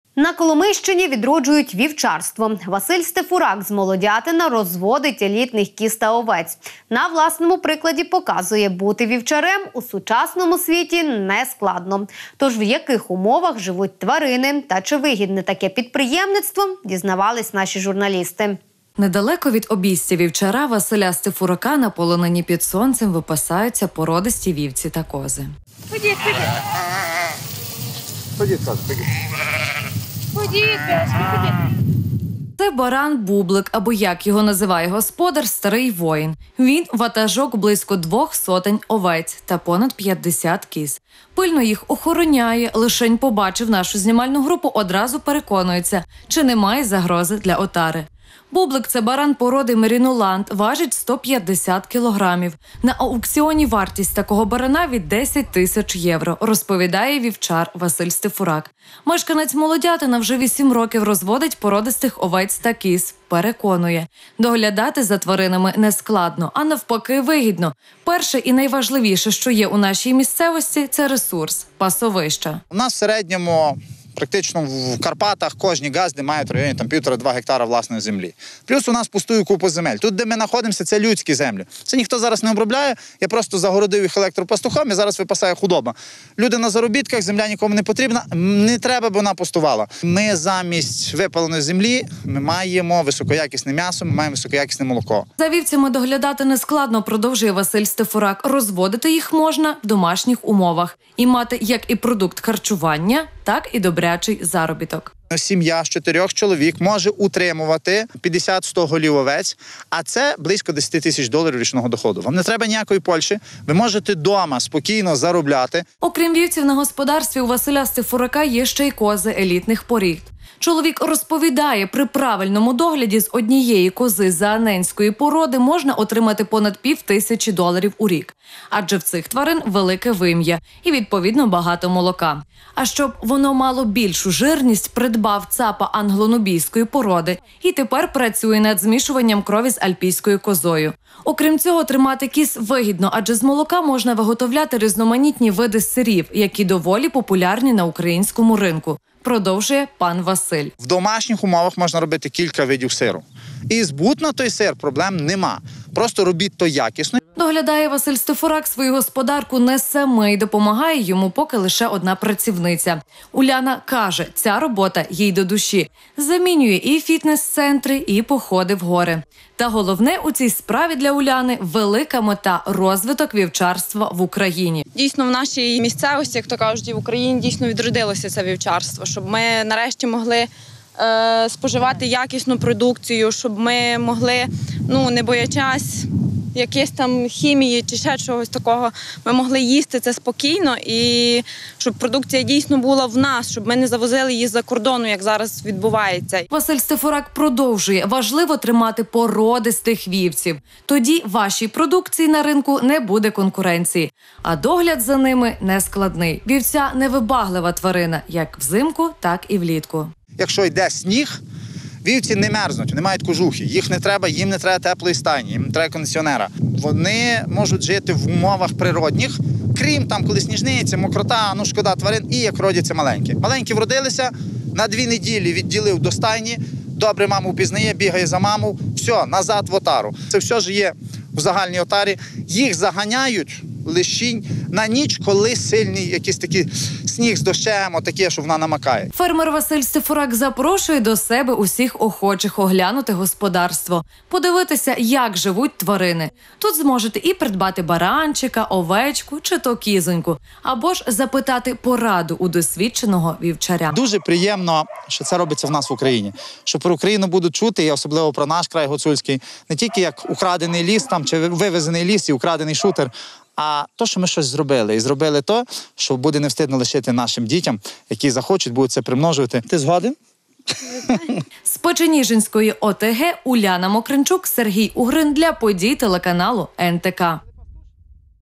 На Коломийщині відроджують вівчарство
На Коломийщині відроджують вівчарство. Василь Стефурак з Молодятина розводить елітних кіз та овець. На власному прикладі показує: бути вівчарем у сучасному світі - не складно, а навіть фінансово вигідно.